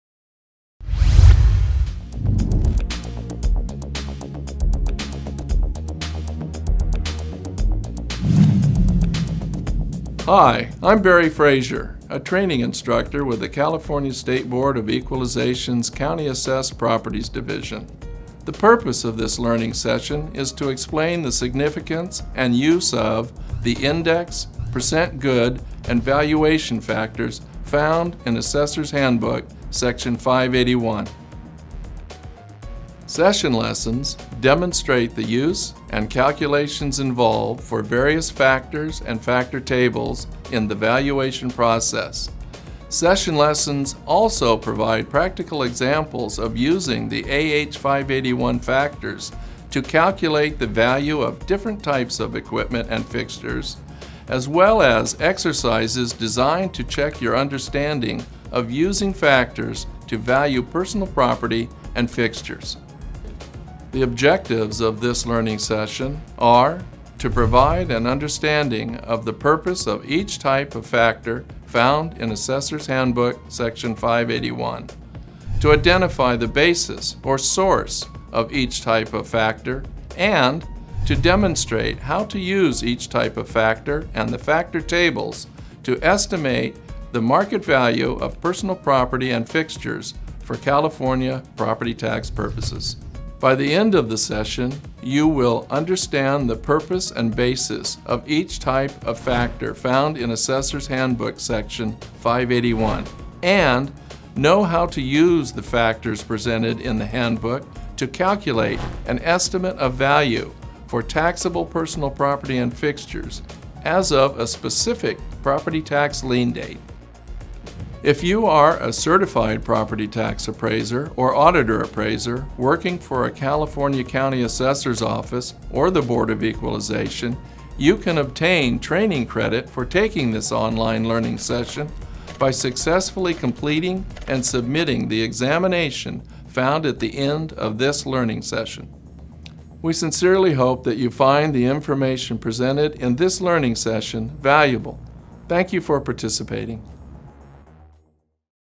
Valuation of Personal Property and Fixtures
The closed captioning for this video is provided as a function of YouTube; as such, the BOE is not attesting to the accuracy of the closed-captioning text. Official meeting minutes and transcripts are available and may be provided upon request.

Welcome to the California State Board of Equalization's self-paced online learning session on the Valuation of Personal Property and Fixtures using Assessors' Handbook Section 581, Equipment Index, Percent Good, and Valuation Factors.

The purpose of this learning session is to explain the factors presented in Assessors' Handbook Section 581, Equipment and Fixtures Index, Percent Good and Valuation Factors, and their function in the appraisal of personal property and fixtures in California.